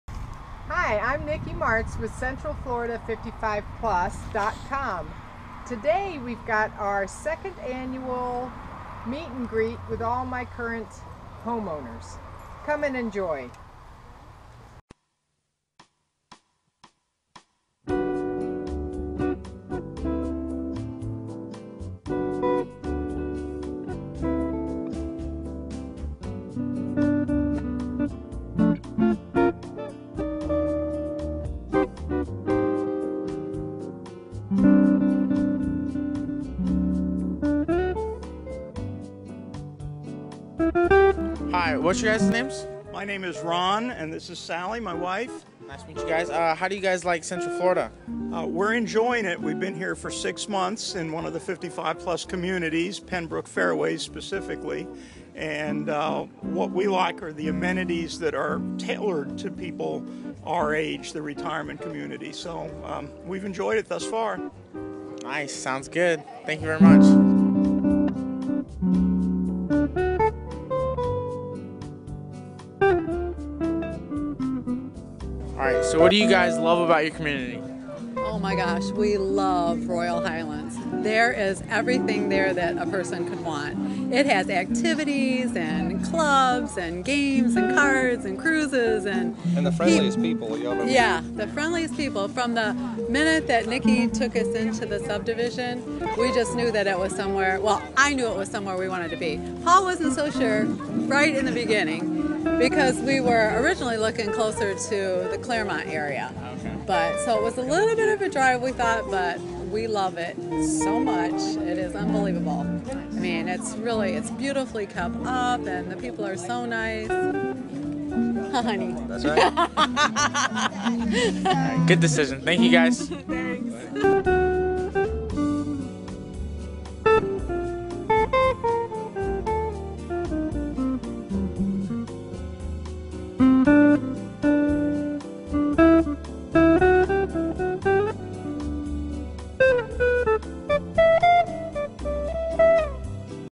2nd annual "meet & greet" @ Yalaha bakery, Leesburg Fl
nicky martyz 2nd annual meet and greet with all of her costumers